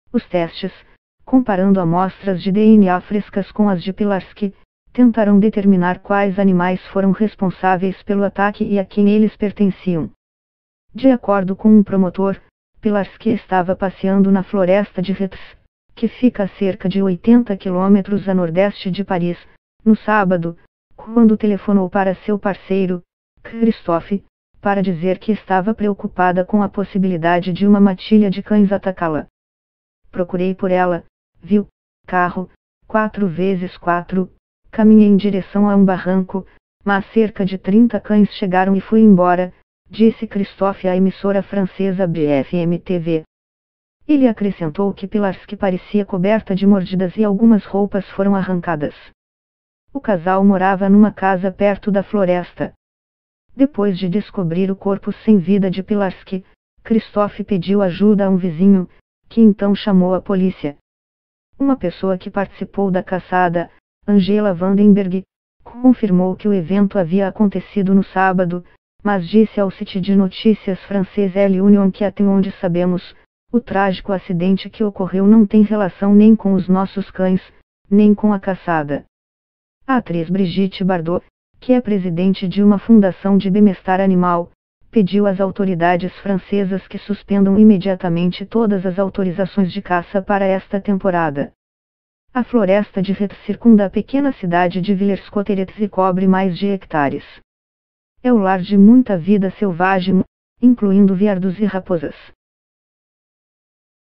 Grávida é Morta Por Cães Durante Caçada Em Floresta Na França
Uma mulher grávida foi morta por cães em uma floresta no norte da França, onde uma caçada estava ocorrendo, dizem investigadores.